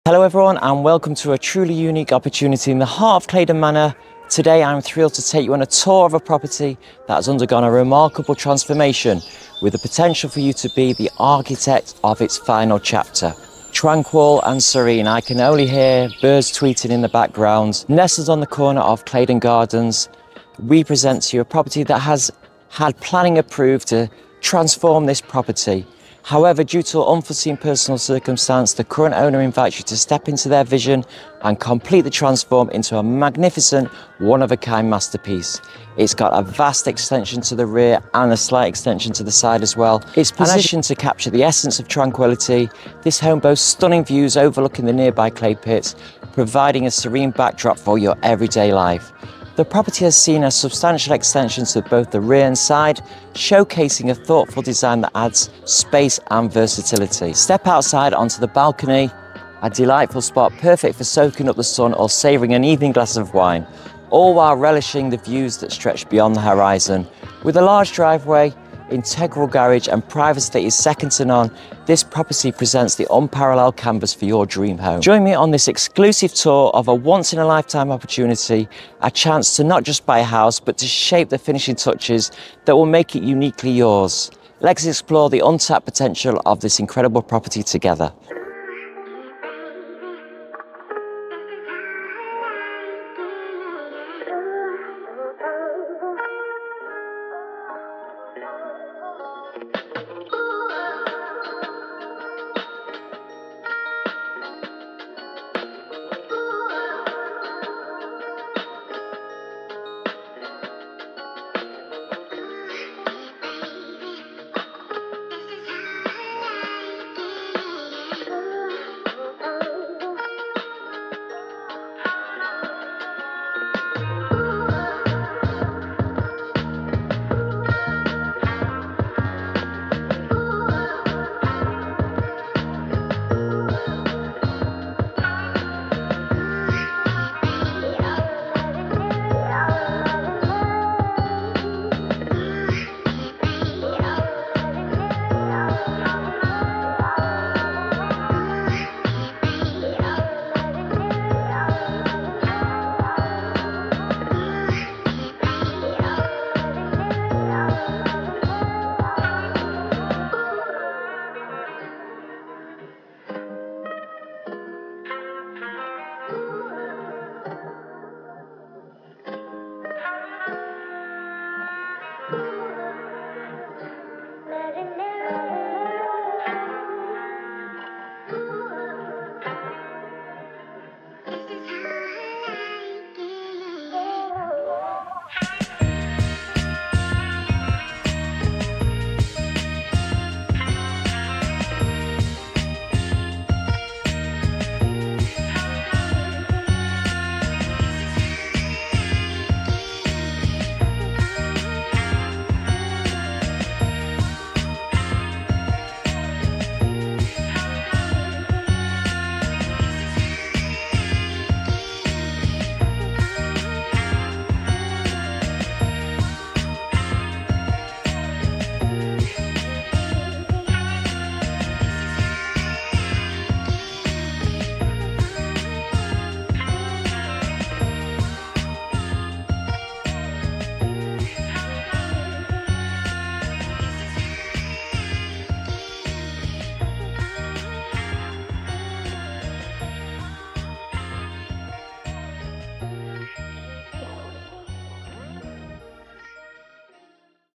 Claydon Gardens, Rixton - Three Bedroom Detached - Property Tour - Large Extension needs completing
Hello, everyone, and welcome to a truly unique opportunity in the heart of Claydon Manor. Today, I'm thrilled to take you on a tour of a property that has undergone a remarkable transformation, with the potential for you to be the architect of its final chapter.
Nestled on the corner of this modern development, we present to you a property that was originally designed as a 3-bedroom detached haven. However, due to unforeseen personal circumstances, the current owner invites you to step into their vision and complete the transformation into a magnificent masterpiece.
Positioned to capture the essence of tranquility, this home boasts stunning views overlooking the nearby clay pits, providing a serene backdrop for your everyday life. The property has seen a substantial extension to both the rear and side, showcasing a thoughtful design that adds both space and versatility.
Step outside onto the balcony, a delightful spot perfect for soaking up the sun or savoring an evening glass of wine, all while relishing the unobstructed views that stretch beyond the horizon. With a large driveway, integral garage, and privacy that is second to none, this property presents an unparalleled canvas for your dream home.
Join me on this exclusive tour of a once-in-a-lifetime opportunity – a chance to not just buy a house but to shape the finishing touches that will make it uniquely yours. Let's explore the untapped potential of this incredible property together."
£500,000

Oliver James
4 Liverpool Road, Cadishead Manchester M44 5AF